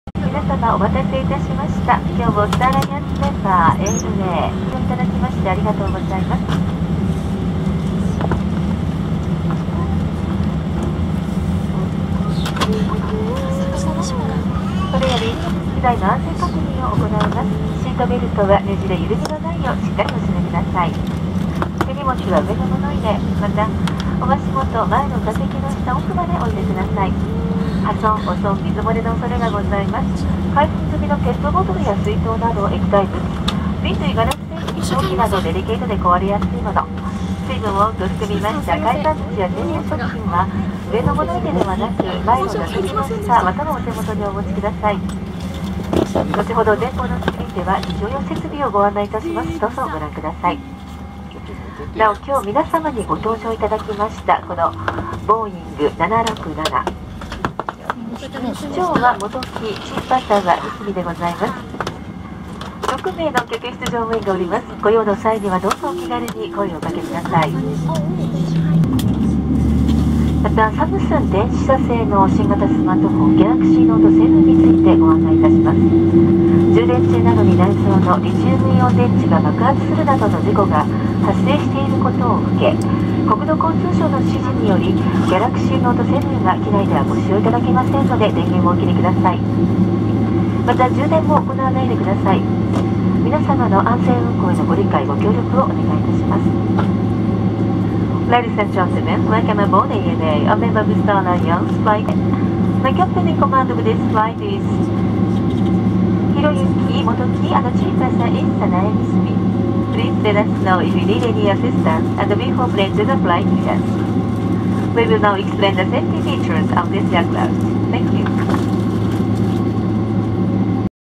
ANA機内アナウンス中、CAが客に必死にお詫び
どうやら、ボーディングの後、滑走路に向けて飛行機が移動中に、座席上の荷物収納ケースをCAさんが閉じているかどうか、確かめていたところ、お客さんの服が落下したようです。
その後のCAさんの神対応が素晴らしかったです。接客のプロフェッショナルさを感じました。
今後も航空業界は、就職戦線で人気であり、CAさんの仕事も

「羽田発ー山口宇部空港行き」
ANA機内アナウンス中、別のCAが客に必死にお詫び